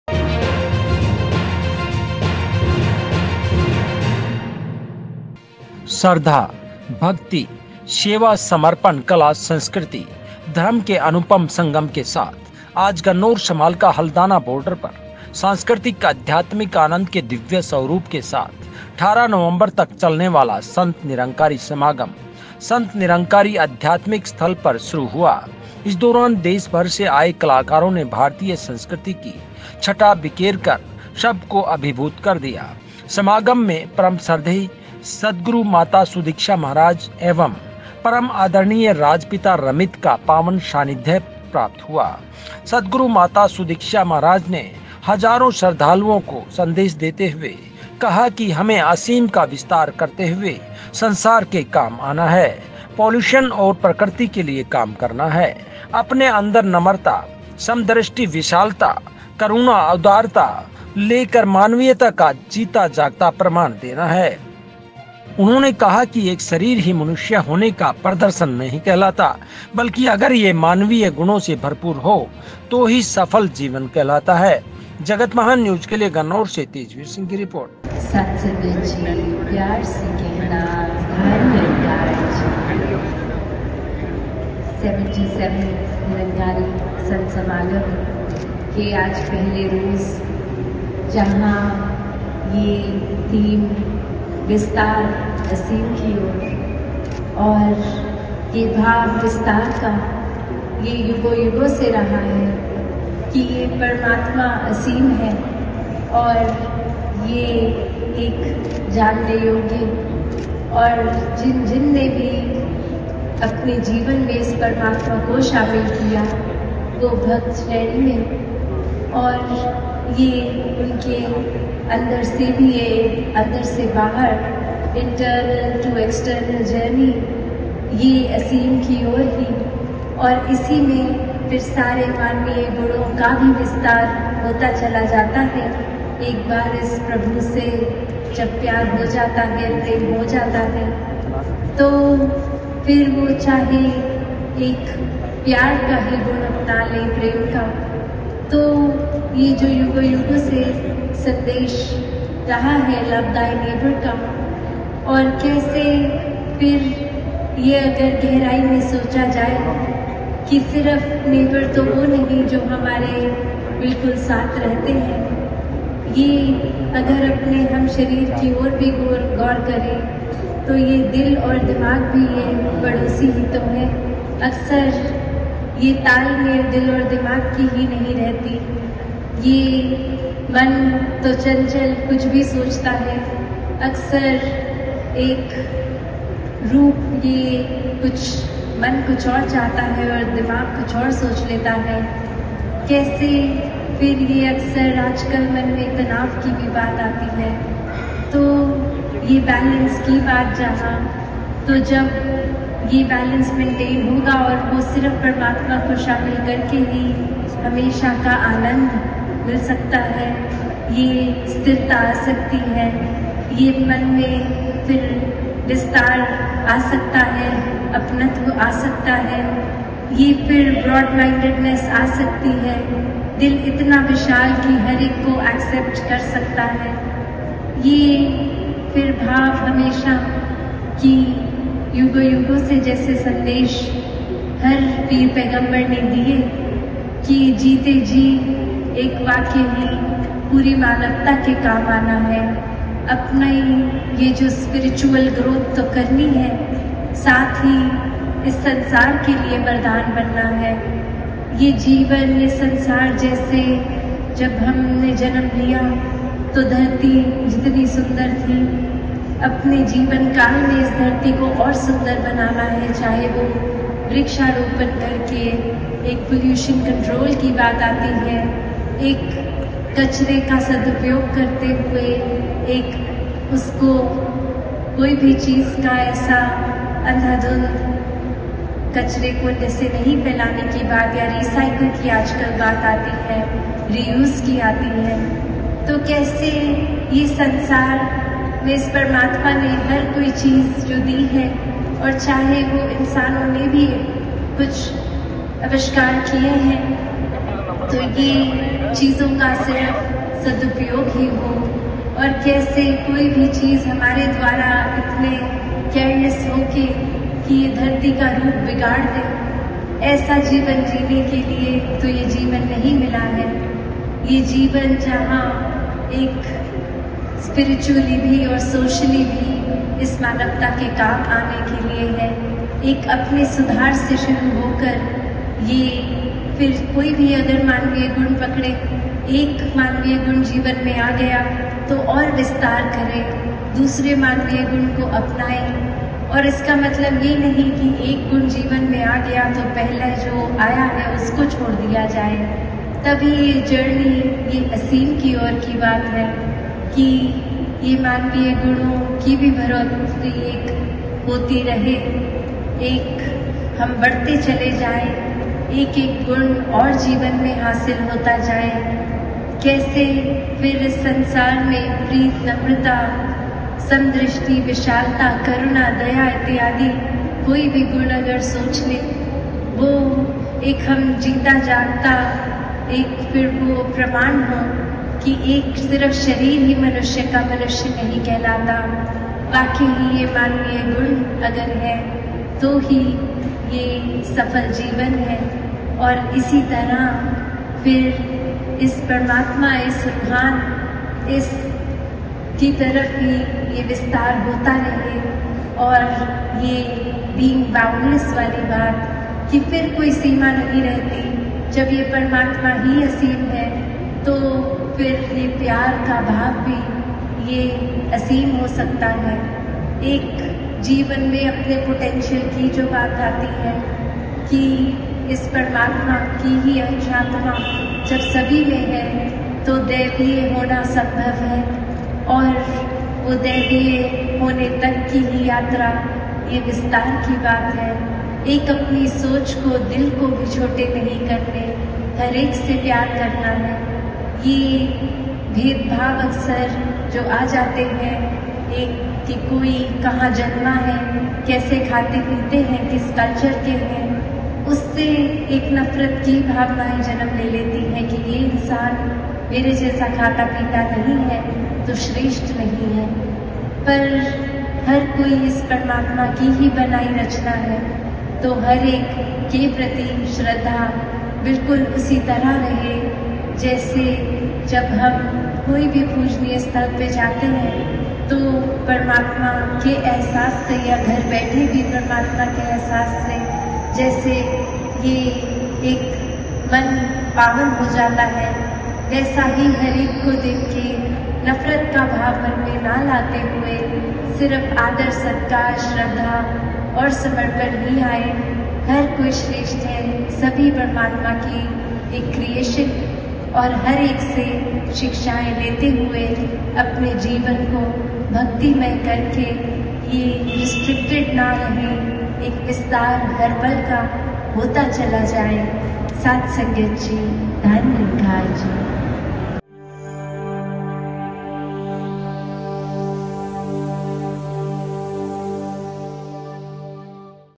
श्रद्धा,भक्ति,सेवा,समर्पण,कला संस्कृति,धर्म के अनुपम संगम के साथ शुरू हुआ 77 वाँ संत निरंकारी समागम
सदगुरु सुदिक्षा महाराज ने दिया मानवता का संदेश। 18 नवंबर तक चलने वाला संत निरंकारी समागम, संत निरंकारी आध्यात्मिक स्थल पर शुरू हुआ !